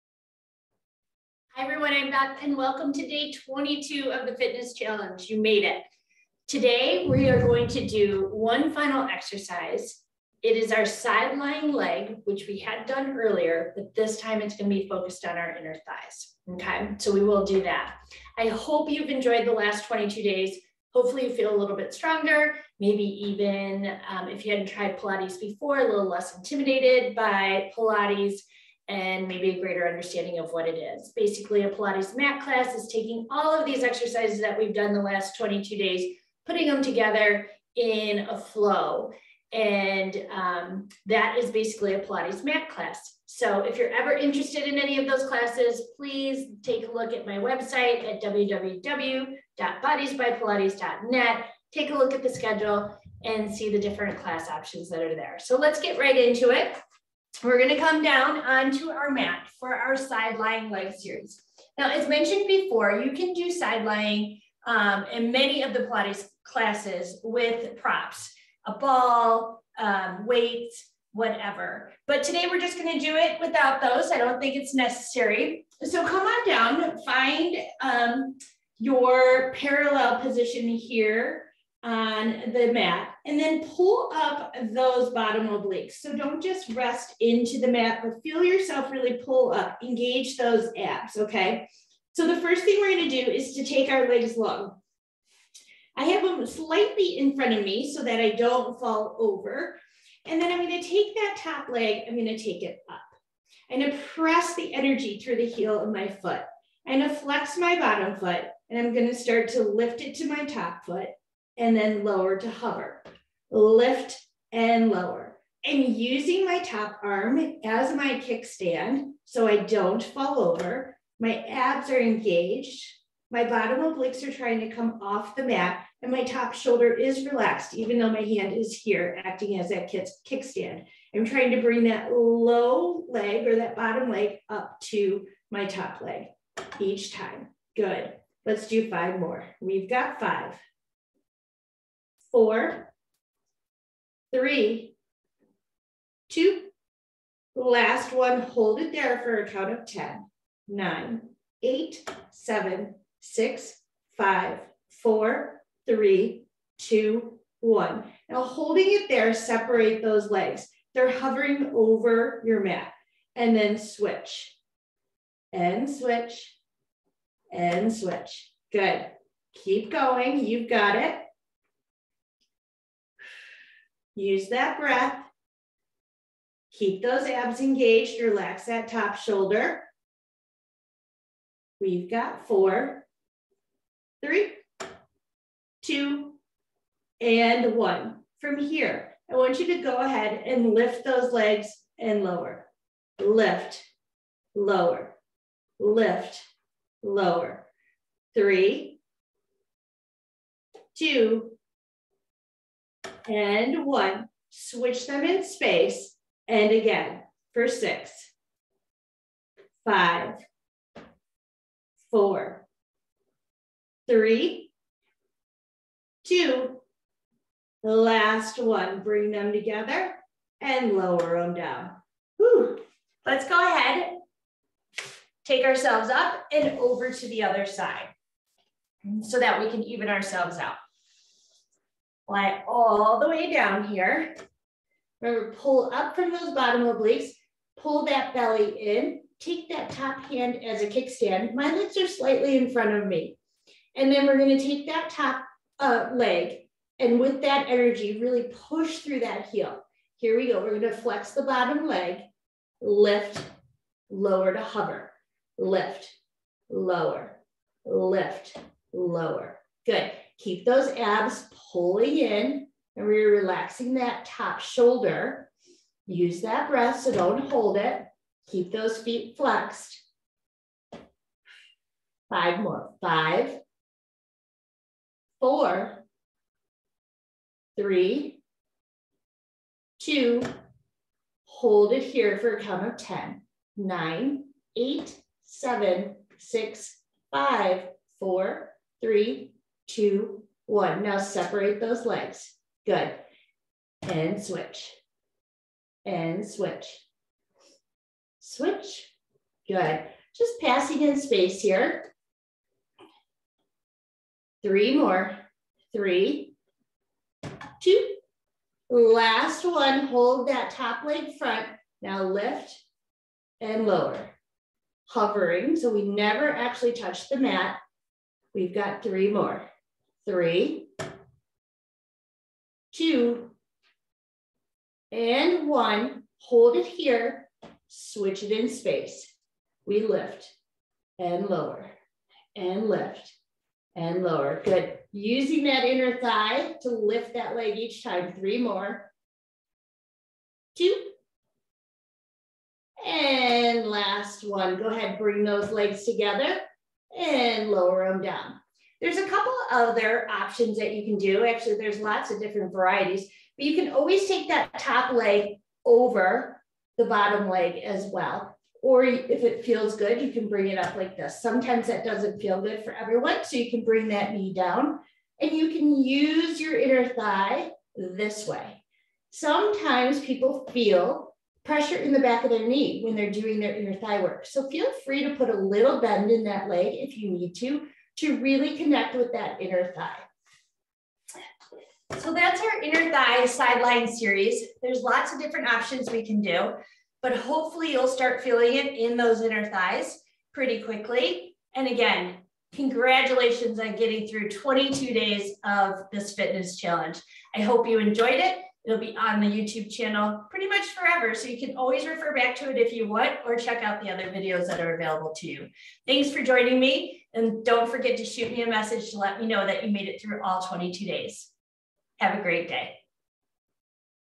Day 22 BBP Fitness Challenge
Hi, welcome to the Day 22, the final day of the Bodies By Pilates 2022 Fitness Challenge!

Today we are going to do a side lying inner thigh series. The inner thigh muscles don't get much of a challenge during normal daily activity, so doing dedicated exercises for them can ensure that they are in balance with the rest of the lower body and core, and are helping stabilize the knee and hip joints. The inner thigh lift also works the abdominals as you set up a solid core to perform the movement. The placement of the top leg can provide an extra hip stretch.